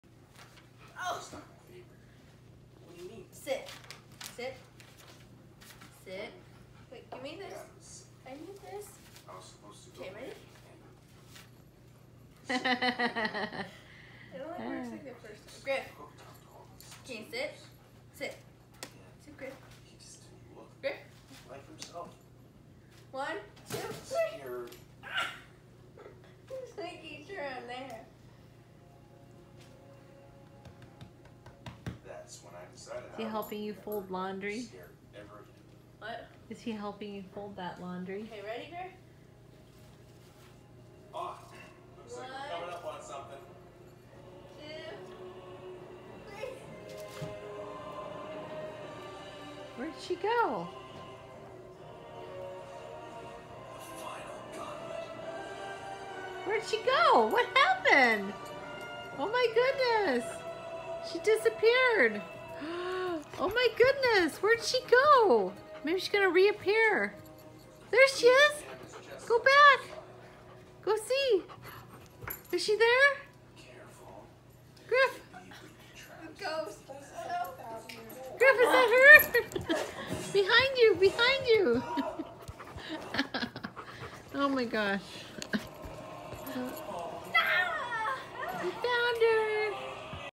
Disappearing behind the blanket 👻
Will Gryff know what happened to Holley?